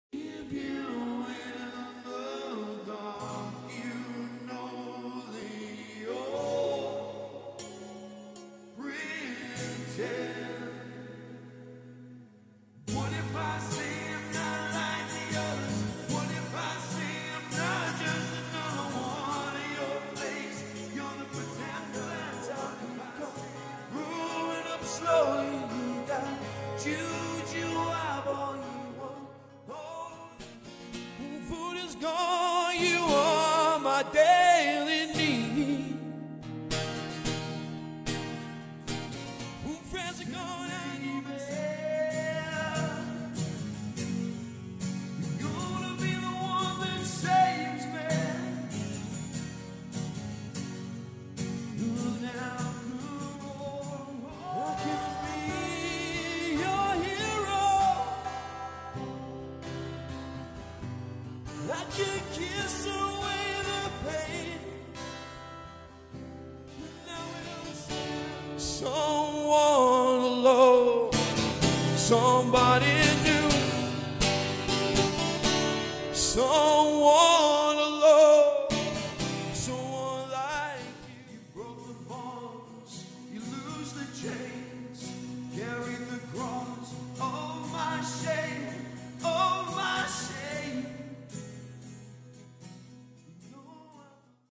Ben Hannington - Short Showreel
Short compilation of cover songs by various artists. Foo Fighters, U2, Beatles, Oasis.